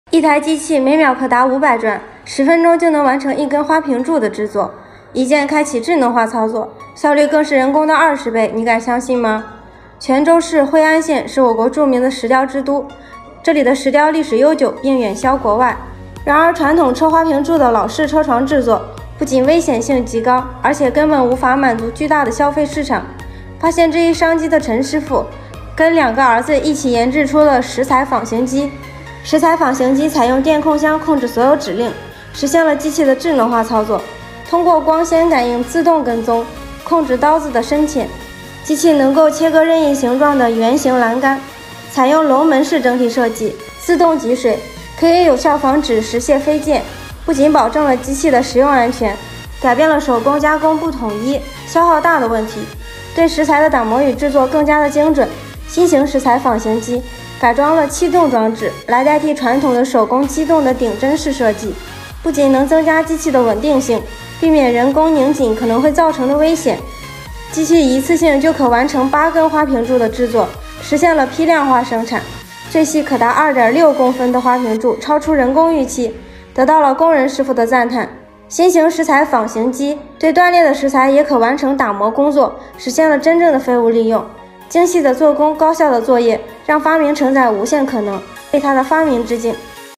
石料界的“雕刻家”？福建牛人發明石材仿形機，精準打磨遠銷國外【發明迷】
惠安的石雕聞名世界，是世界的雕刻之都，但是在生產加工過程中，會產生出大量廢棄的石頭，不能很好的利用起來。另一方面以前的石材加工都是靠手工，工人在加工過程中深受環境污染跟灰塵困擾，不僅對身體不好，而且效率非常的低。為了能夠解決和改善這方面的問題，來自福建惠安的陳錫奎父子三人，一起做出了一台石材仿形機。這台機器真正的實現了全自動操作，由原先的手動操作改成了氣動，全程操作都不需要人工來干預，非常的安全。
發明迷 石材仿形機 石材機 请放心，本视频内容符合YouTube社区准则，没有任何违反规定的行为。